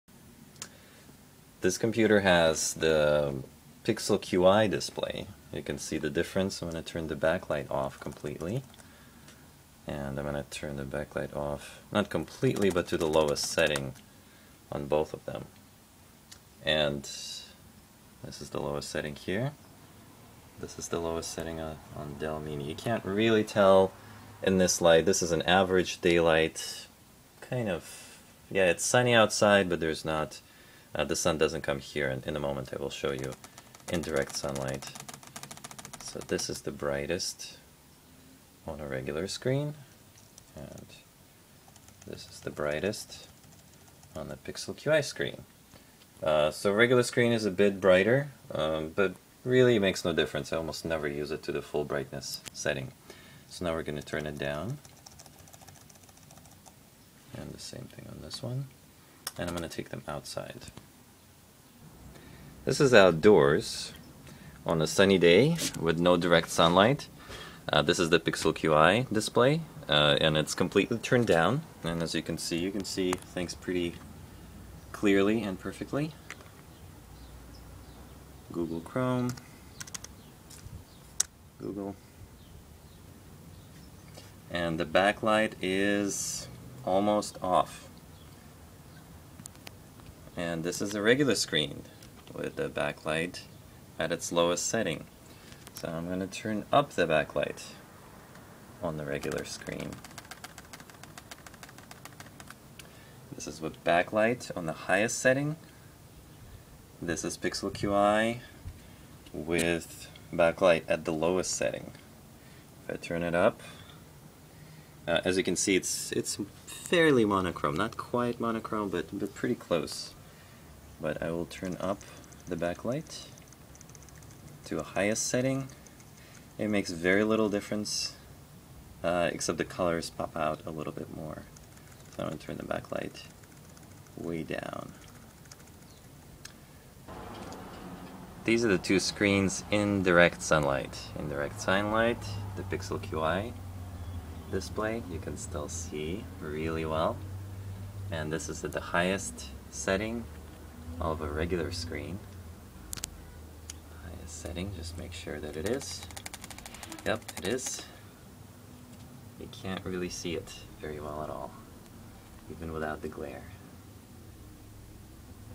Pixel Qi vs Regular Netbook Screen. Indoors, outdoors, in the sun.
Pixel Qi on Dell Mini 10v vs Regular Netbook screen on Asus 1005HAB.